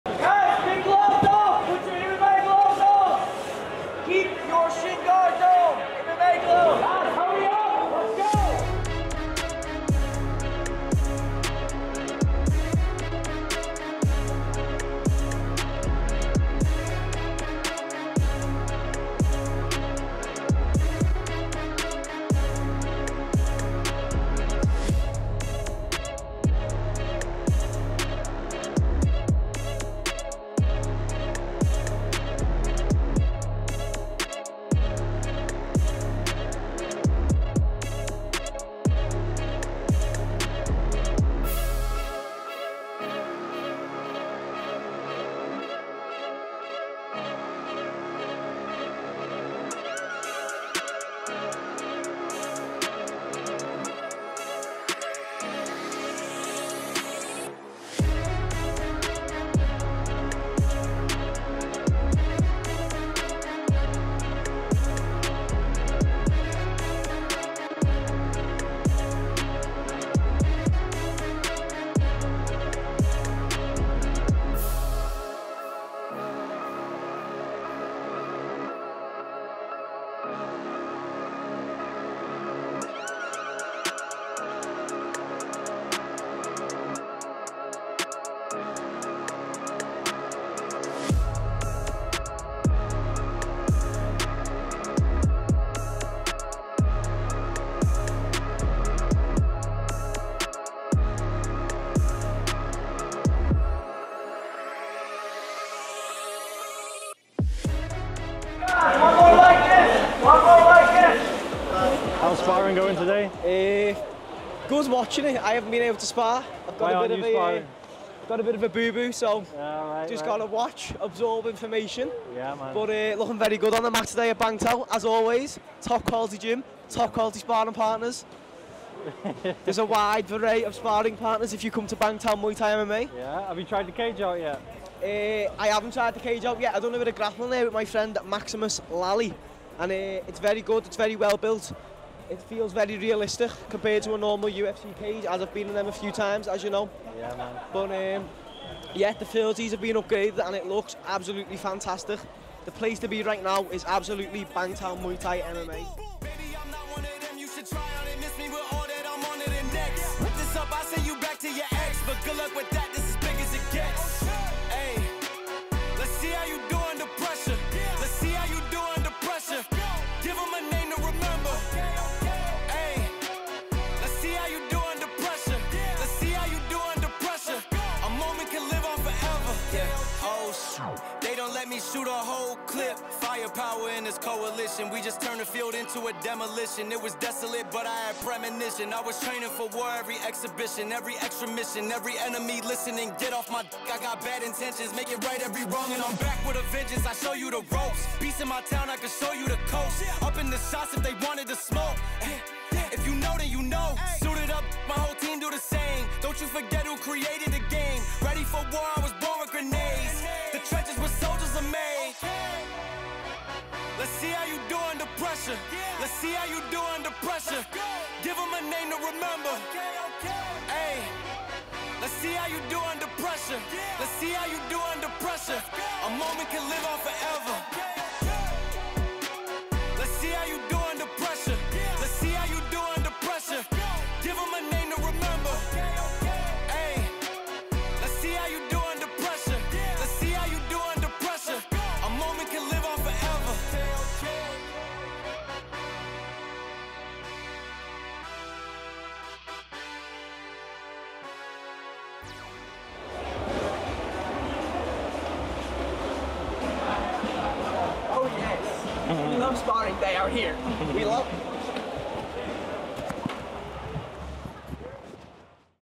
MMA Sparring | Class Highlights | George Hickman | Bangtao MMA
Highlights from Fridays MMA Sparring Class. Join us here at Bangtao MMA Mondays & Fridays for MMA Sparring